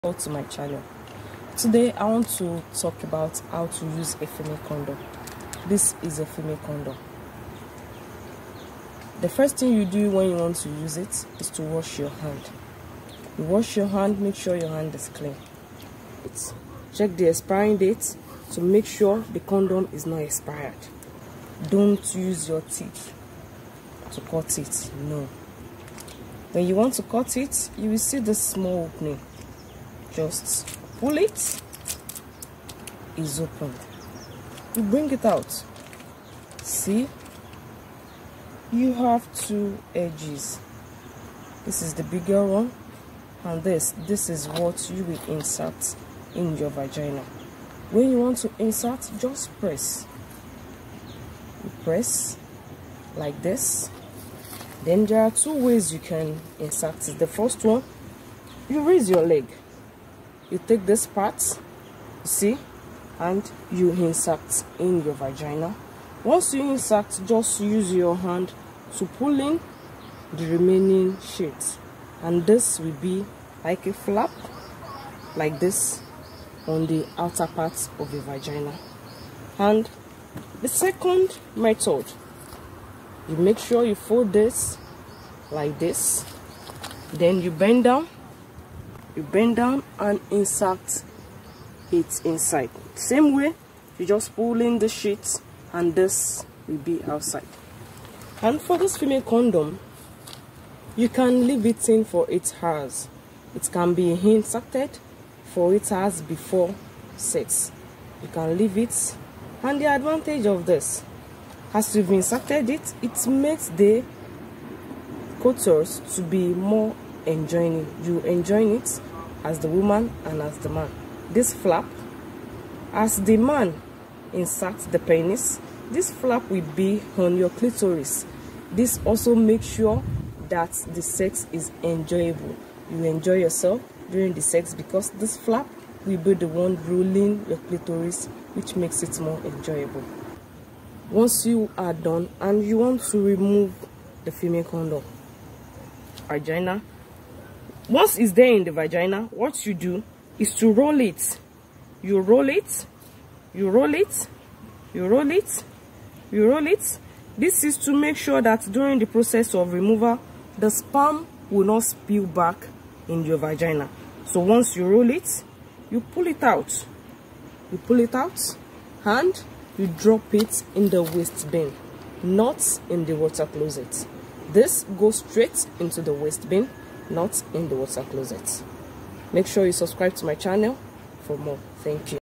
How to insert a female condom
Female condoms are made from soft, thin synthetic latex or latex. They're worn inside the vagina to prevent semen from getting to the womb.

Female condoms serve a dual purpose, it prevent pregnancy as well as sexually transmitted diseases.

For women, a female condom makes sex more enjoyable as the flap in the outer part of the vagina will rub the clitoris during coitus.

You can share your experience in the comment section and let me know if you will try it.